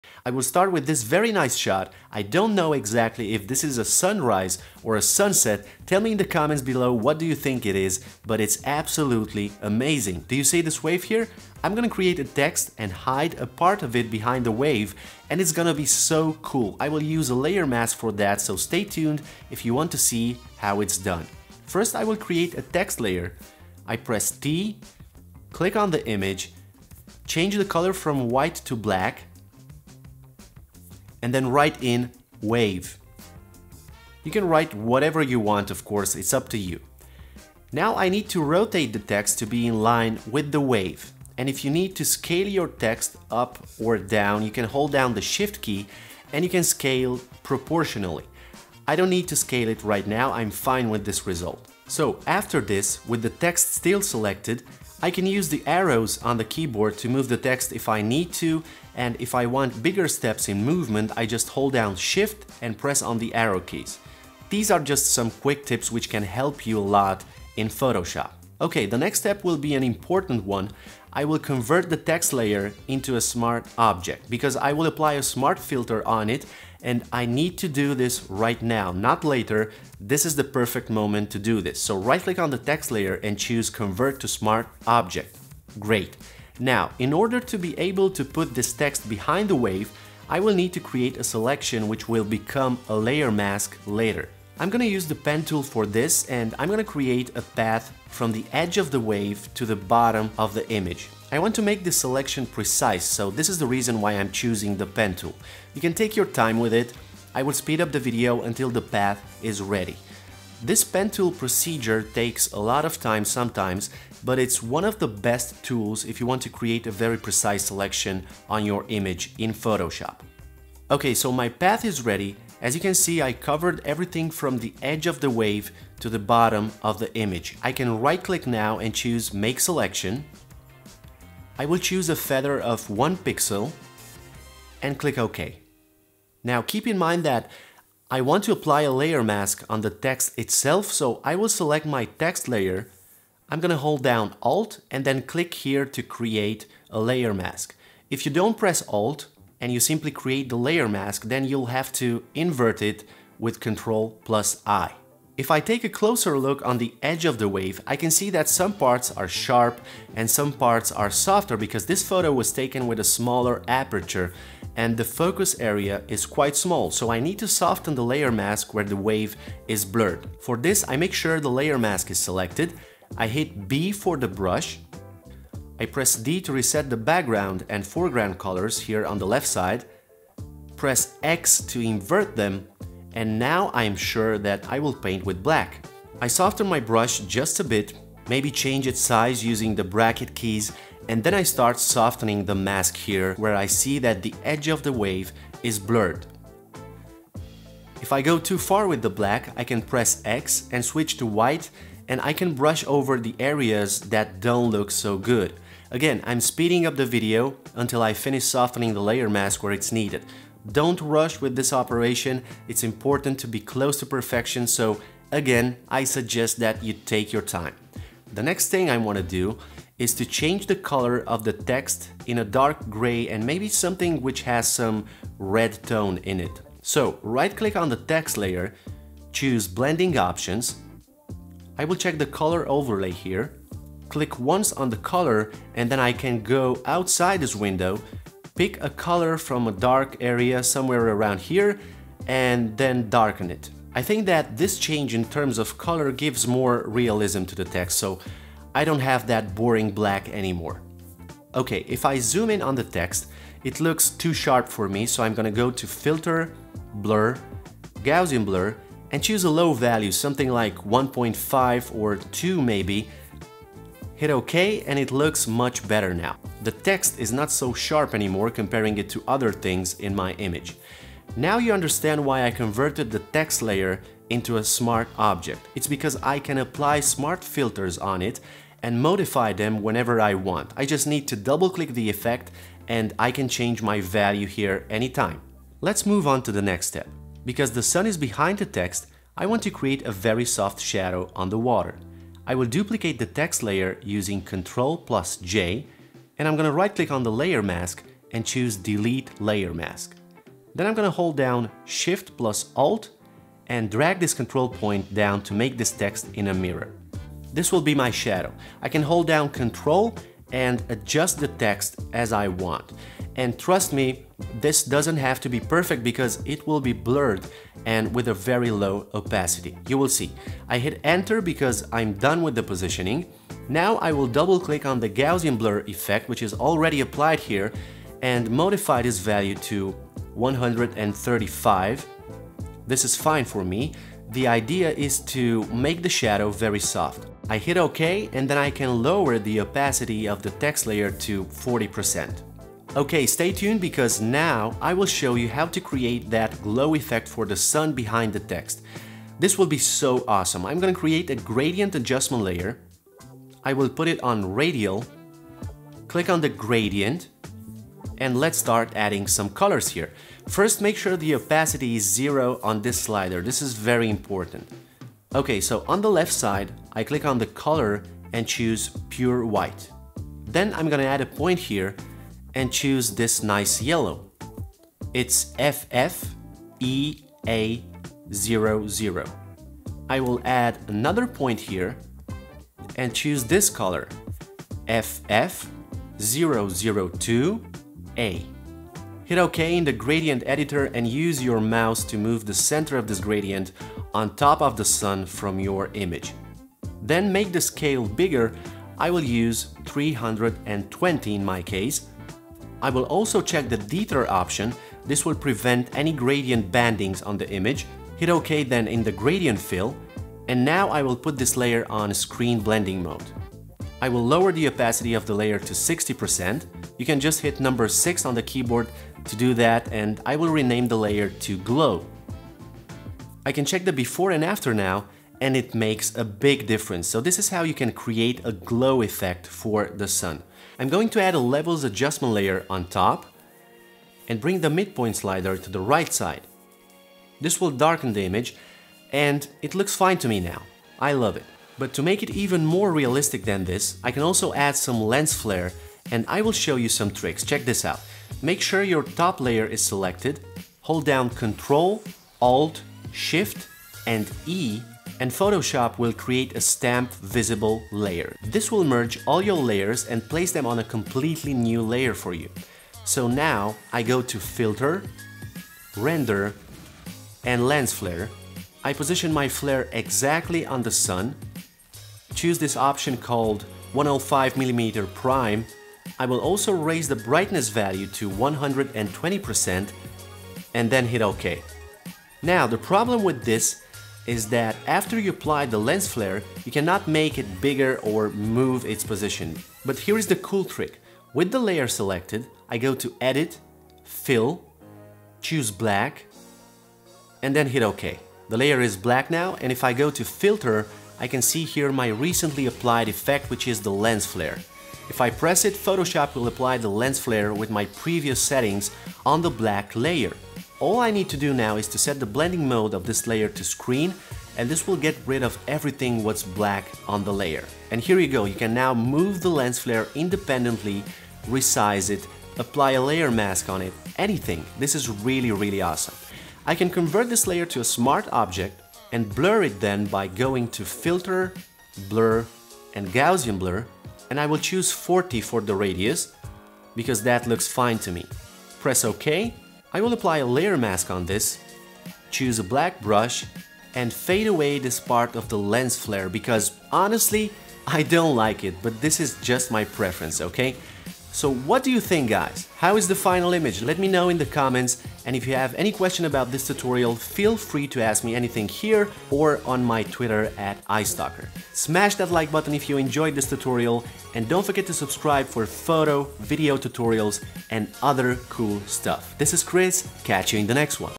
How to Put a Text Behind an Object: Photoshop Tutorial
In thisPhotoshop tutorial, you'll learn how to place text behind an object.